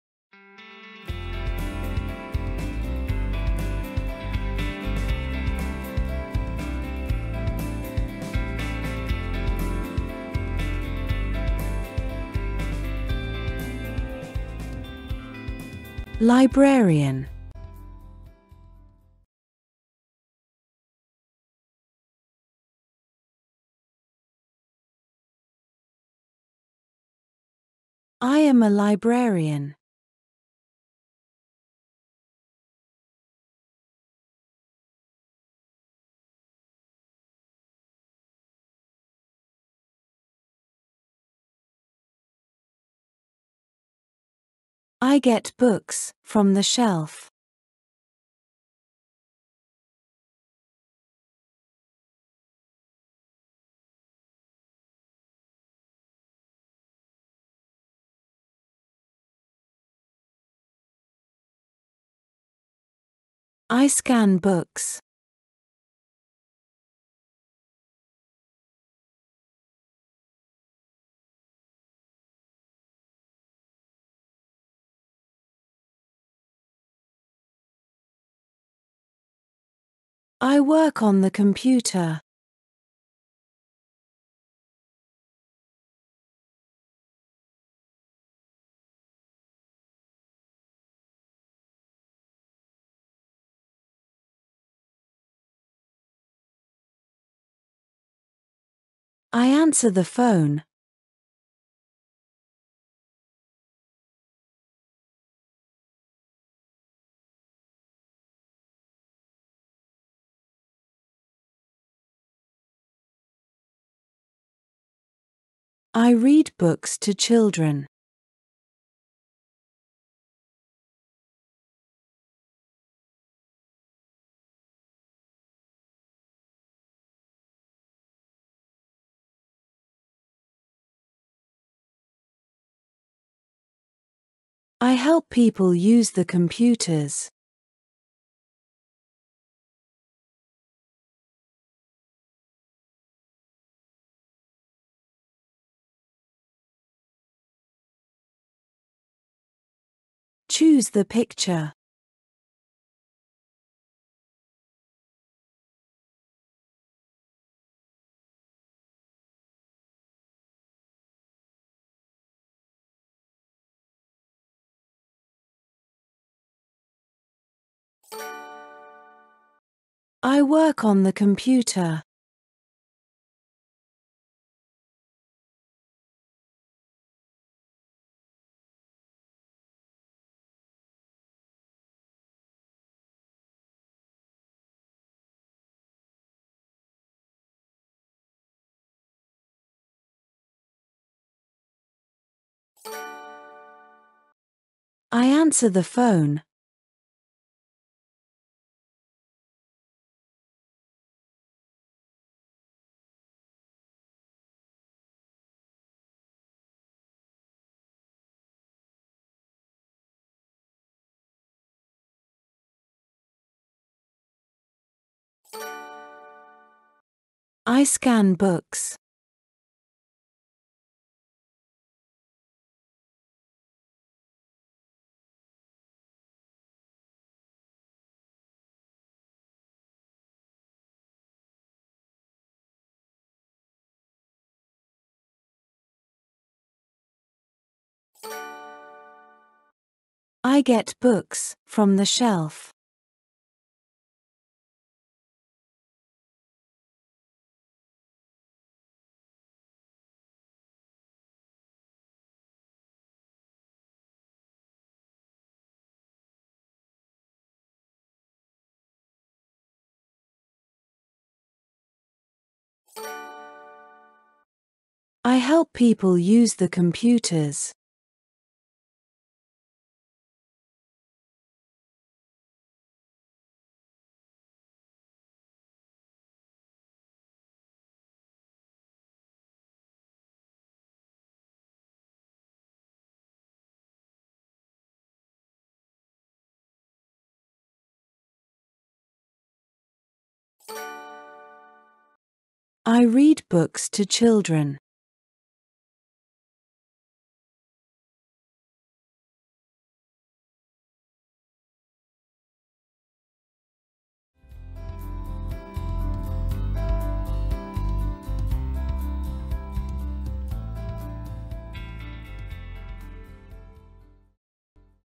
Librarian (My Busy Day)
Easy English - Beginner English - A1 English - Jobs - Daily Routine - AMEP

This video can be used with the English Express Beginner Workbook.

for English learning resources.